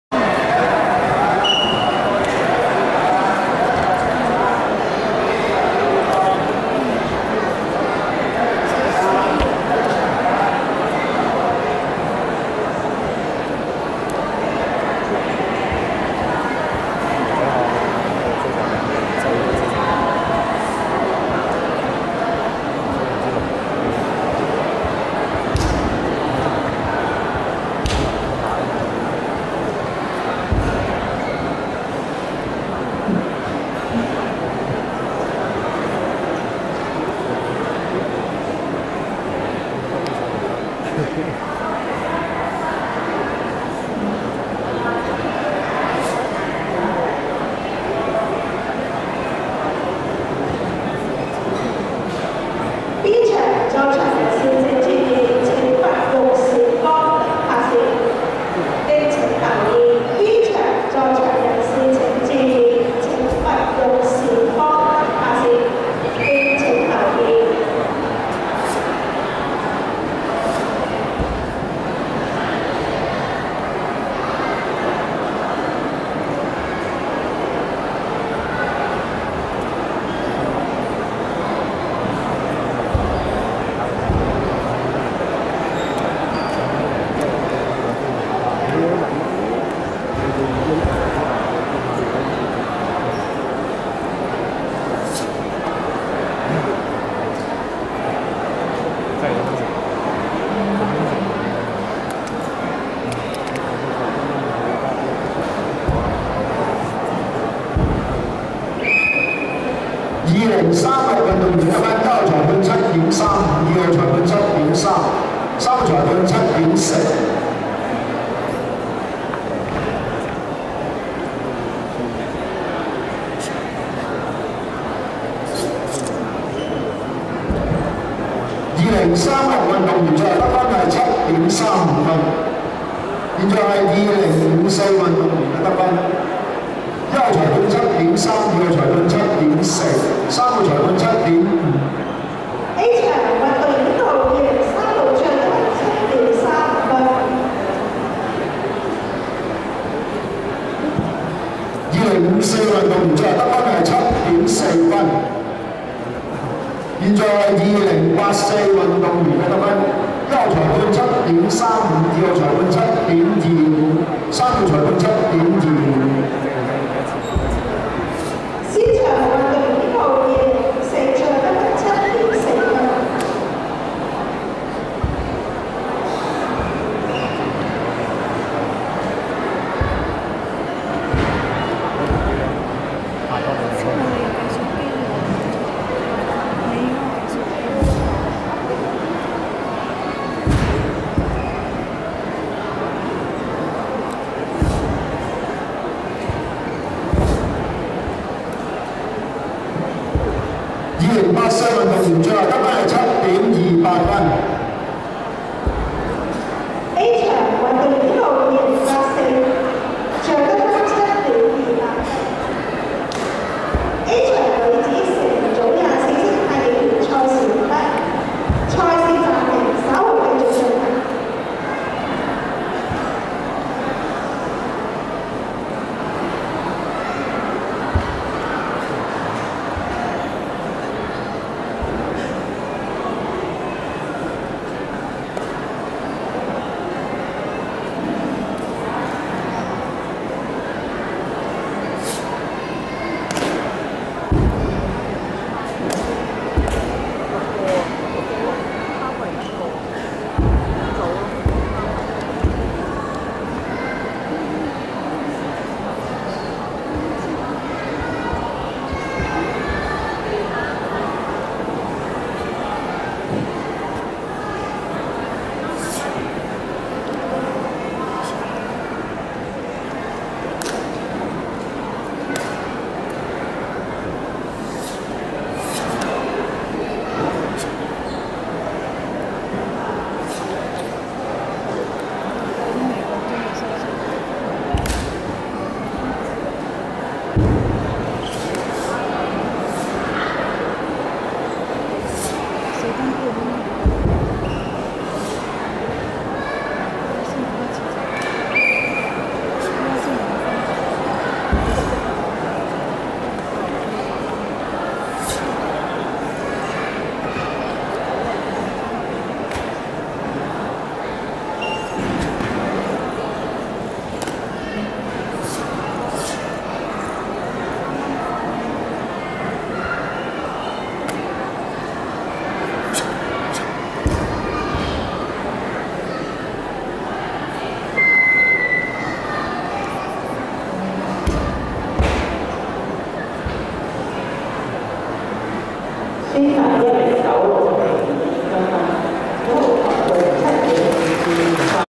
中華太極社 2014武聯 全港公開太極錦標賽 陳式56太極拳 - 何季平 (優異獎)
中華太極社 China Taichi Club / Zhonghua Taiji Club
馮軍利師傅 . Master Feng Junli